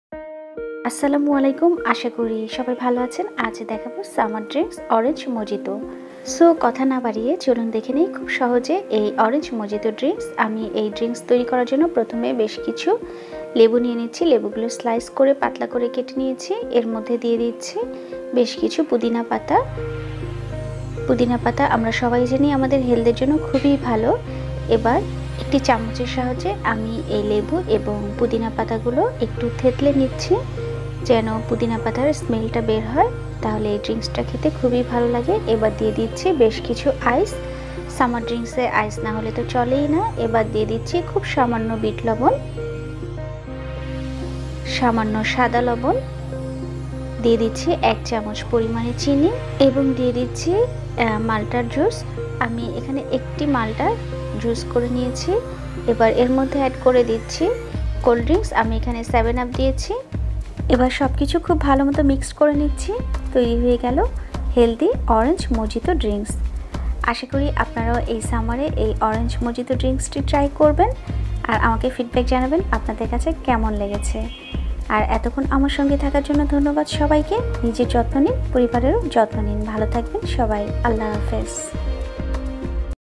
Orange Mojito || কমলার জুস || summer drinks || orange juice || orange mocktail
Hello everyone wellcome to hobby Daily. Today i present the recipe of Orange mojito, কমলার জুস and most common  Summer Drinks.  Hope u all love that Orange Mojito ||  কমলার জুস Its a healthy summer drinks & so easy to make. Be healthy and Keep heathy always. Very simple ingredients are to be needed.

Ingredients :
1. Orange / malta
2. Lemon
3. Fresh mint leaves
4. Black salt
5. White salt
6. Sugar
7. Ice cube
8. Soda/ 7 up/ sprite